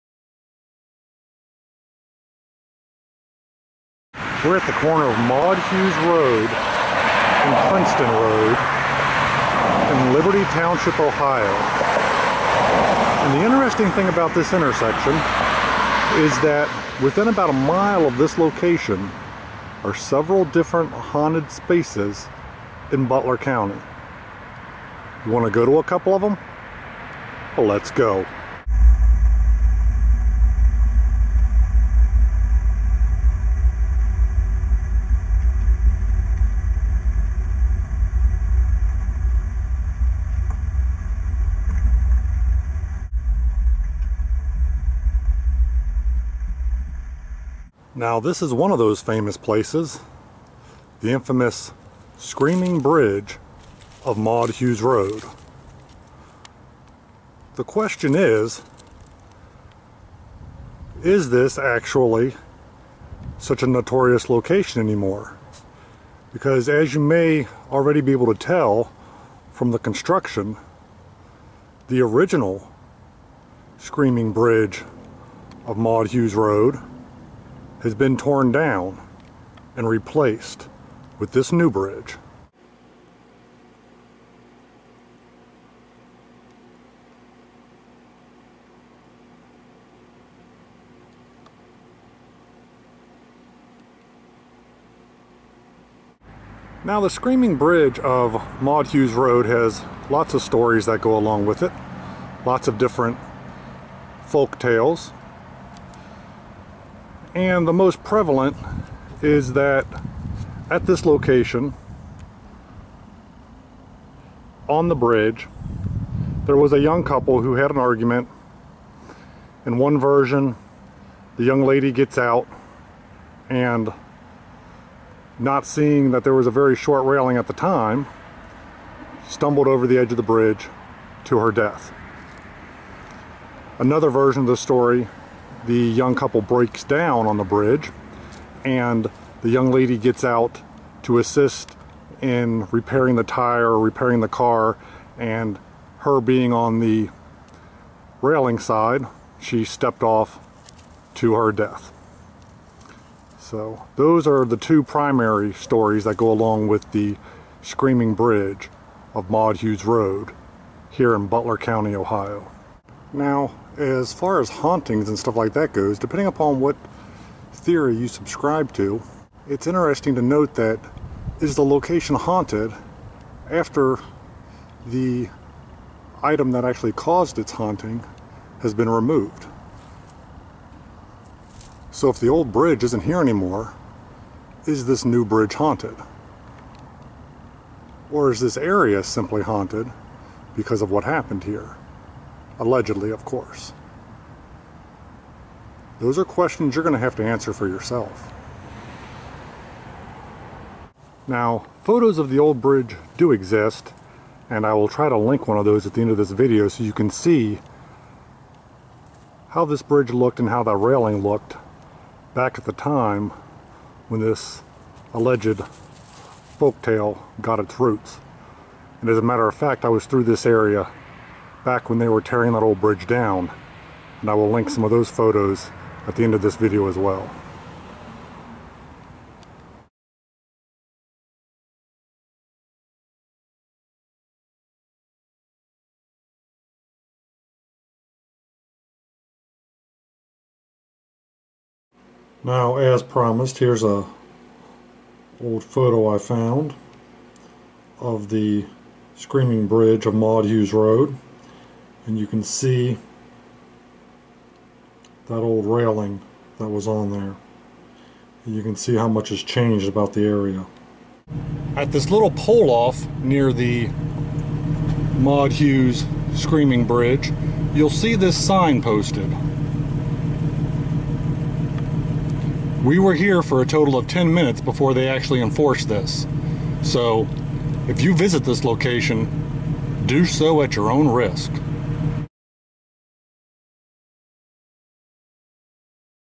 Haunted Screaming Bridge of Maud Hughes Road - Liberty Township, Ohio
Come along for a look at the "haunted" Screaming Bridge of Maud Hughes Road - Liberty Township, Ohio.

If you decide to visit this location it is clearly posted "No Trespassing." The woods are owned by a private party and the tracks are owned by the railroad. This area is MONITORED! Stop and trespass at your own risk!